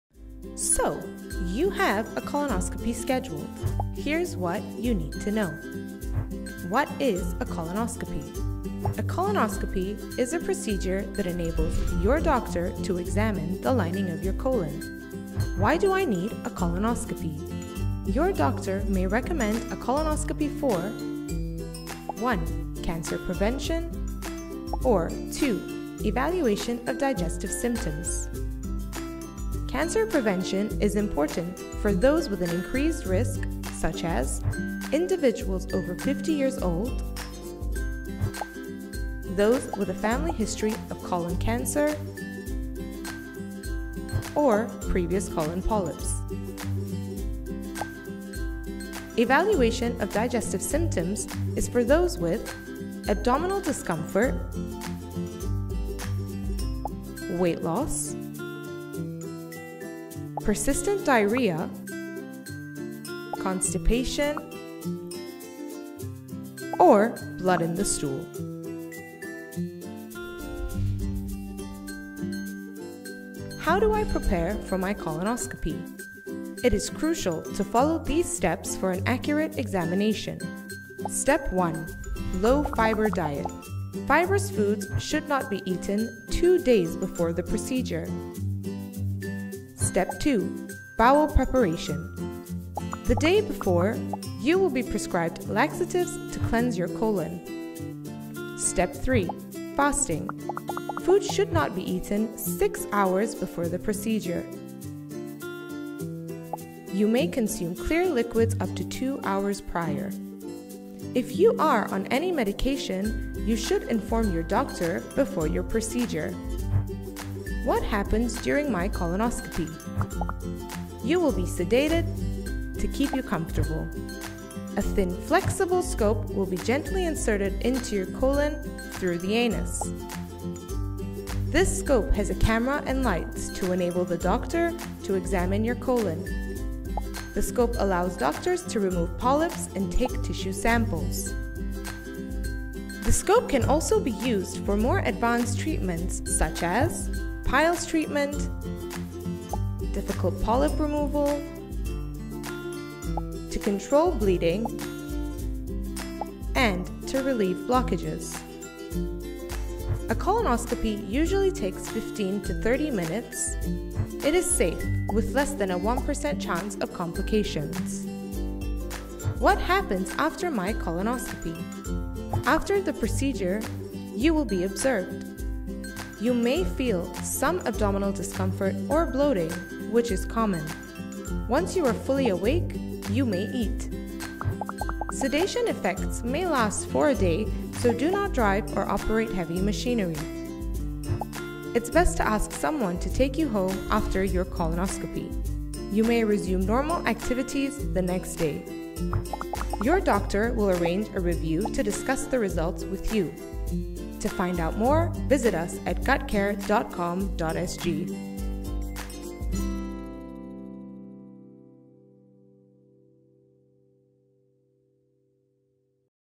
Persiapan Untuk Kolonoskopi Anda (Colonoscopy) | GUTCARE
Kolonoskopi adalah prosedur untuk melihat ke dalam usus besar anda dan memeriksa lapisannya. Ahli gastroenterologi yang akan melakukan kolonoskopi ini.

Alat utama yang digunakan untuk melihat ke dalam usus besar adalah kolonoskop, yang merupakan tiub lembut dan fleksibel yang juga panjang dan nipis. Kolonoskop dimasukkan ke dalam dubur dan ianya cukup panjang untuk melihat seluruh usus besar dan bahagian bawah usus kecil. Tiub ini mempunyai kamera kecil dan cahaya di hujungnya untuk membolehkan tayangan prosedur secara langsung dan gambar usus besar untuk dipaparkan di skrin. Pemeriksaan dilakukan selepas penenang diberikan, supaya pesakit tidak sedar semasa prosedur.